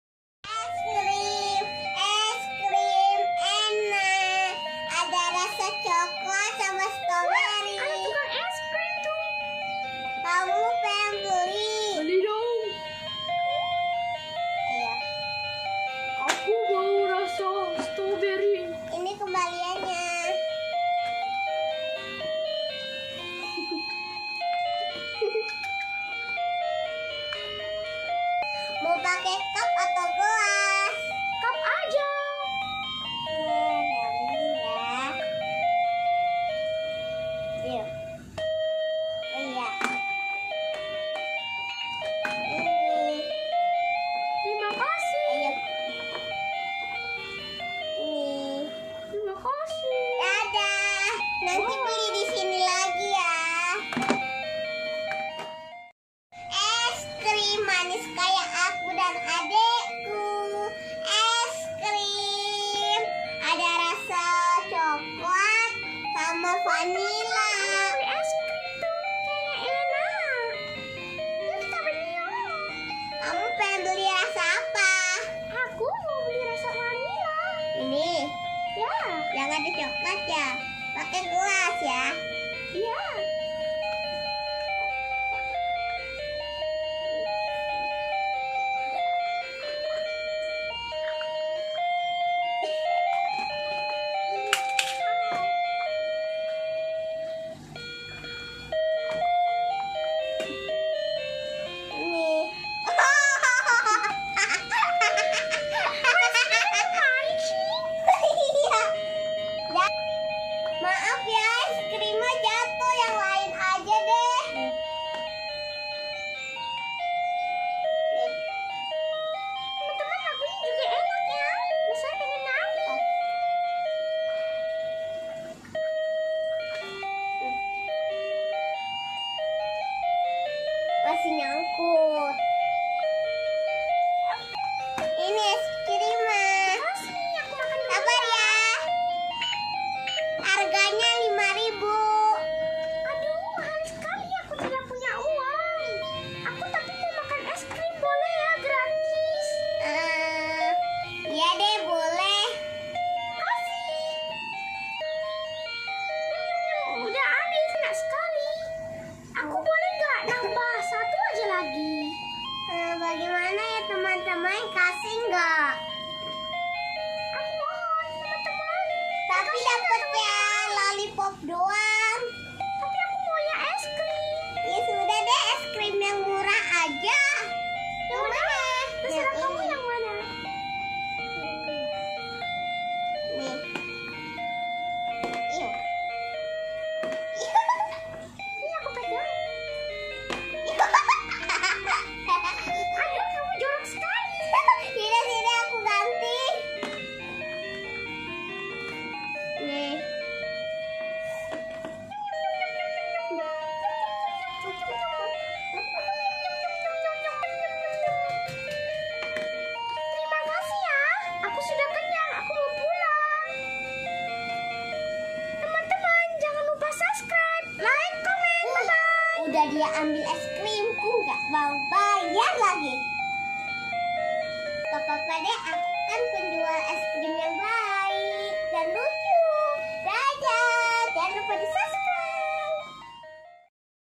Bermain/menjadi penjual ice cream yang baik dan lucu
di video kali ini ariladanyohana bermain menjadi penjual ice cream yang baik dan lucu.

yuk teman teman nonton sampai habis yaa
dan jangan lupa bantu like dan subscribenya.terima kasih.

           ariladanyohana😊😊😉